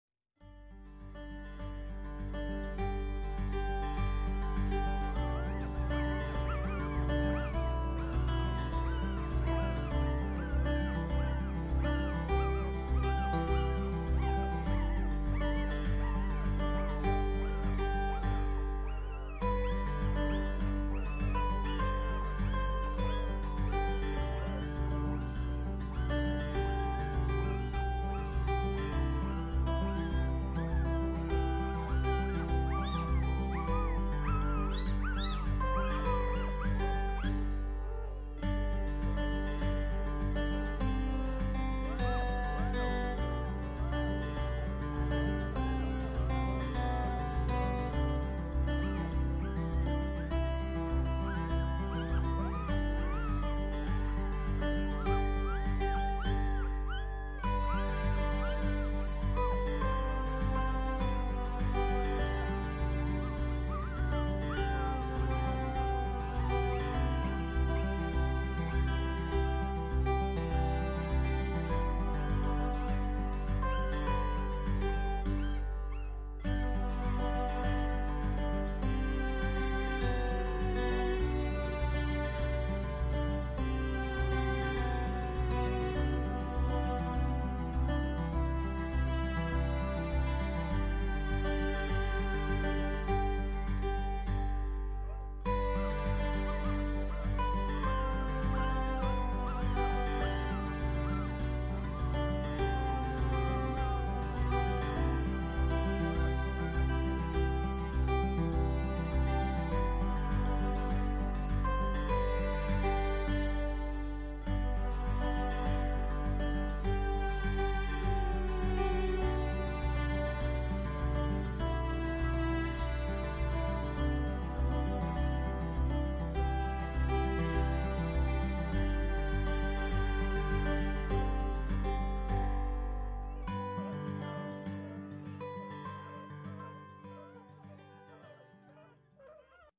Mayla's New Born Litter
New Born F1 Goldendoodle puppies, tamarackridgegoldendoodles.com